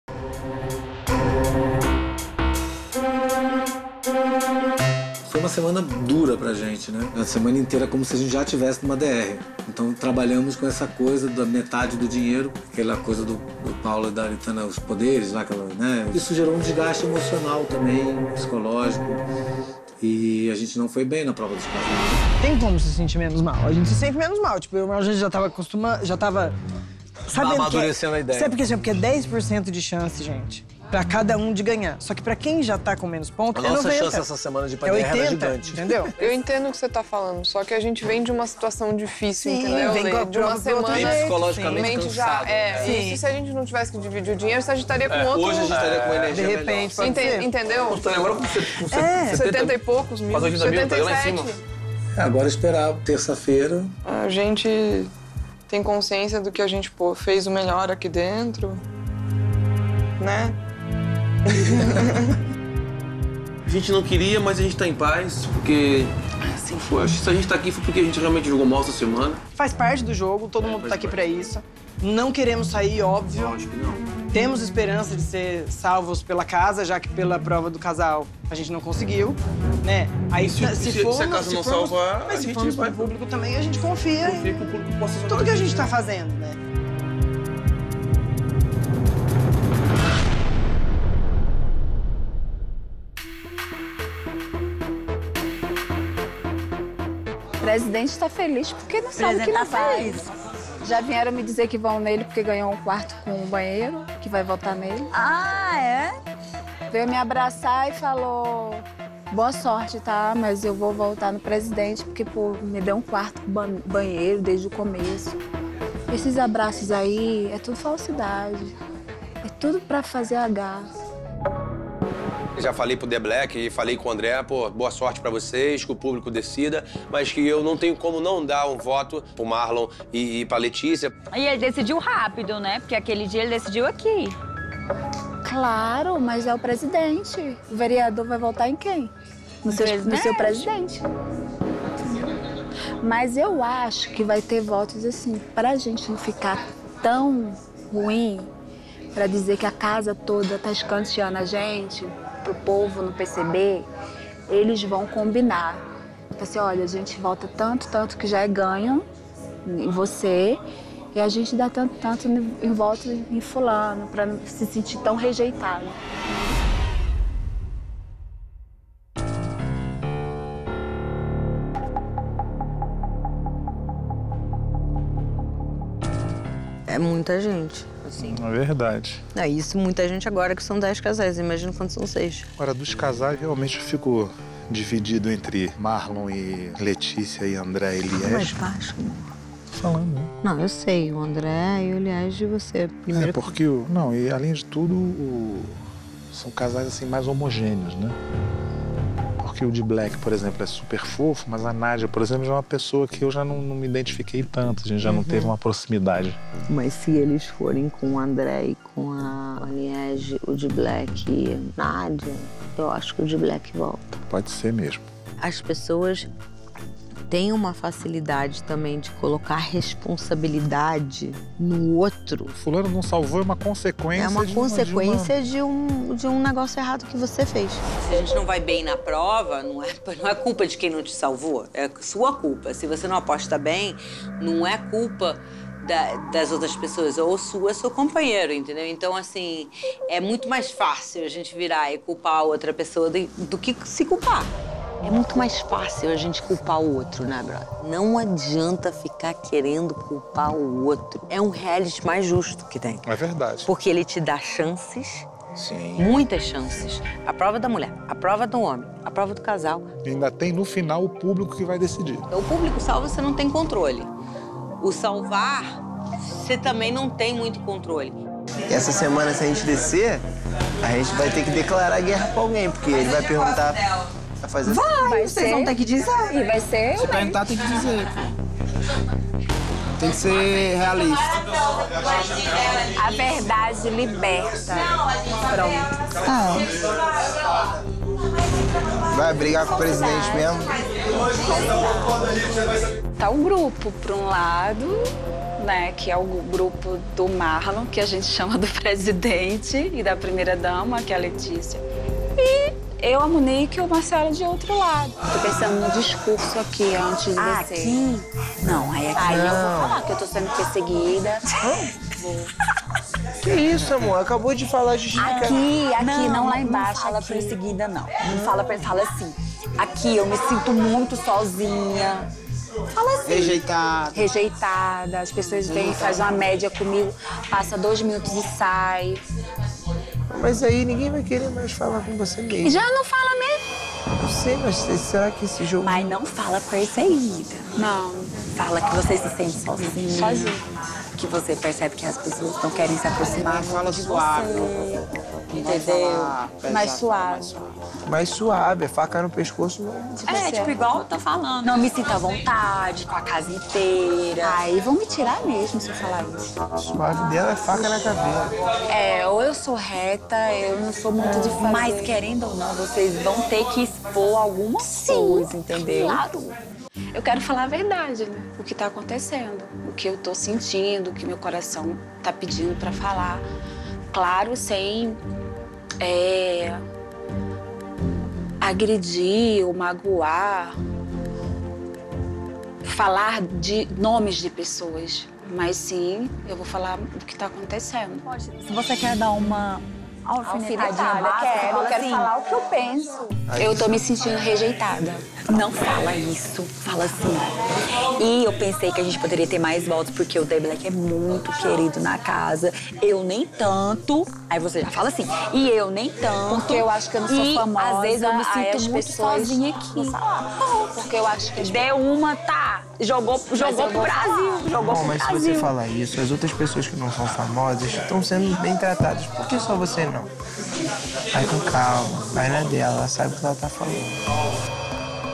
Casais da DR especulam votos que podem tirá-los da zona de risco
Letícia e Marlon, Liége e André e Nadja e D'Black estão na DR, mas um deles será salvo pela votação dos casais da mansão. Enquanto a eliminação não chega, eles tentam entender como cada um deve votar.